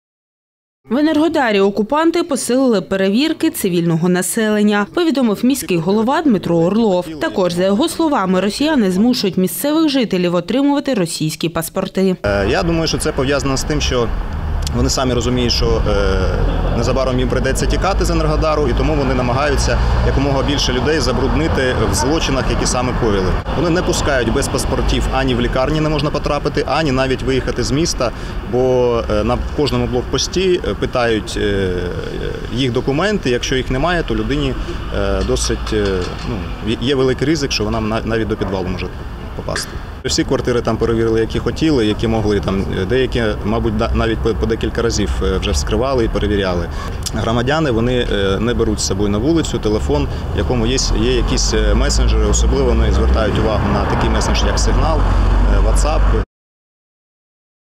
У тимчасово окупованому Енергодарі росіяни посилюють перевірки місцевих | Новини
В Енергодарі окупанти посилили перевірки цивільного населення – повідомив міський голова Дмитро Орлов. Також, за його словами, росіяни змушують місцевих жителів отримувати російські паспорти.